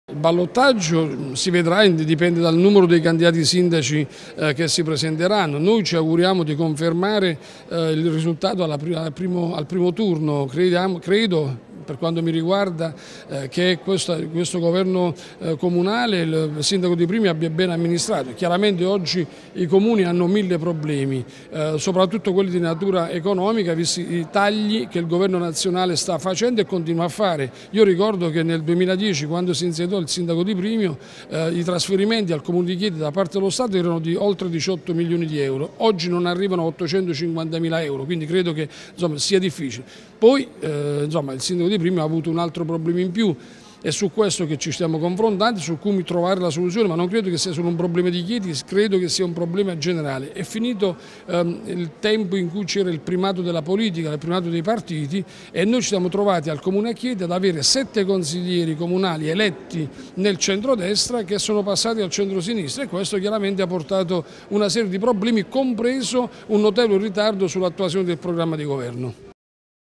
Febbo su amministrative Chieti
L'ex assessore regionale Mauro Febbo rilancia sulla possibilità di effettuare le primarie per la scelta del candidato sindaco del centro destra a Chieti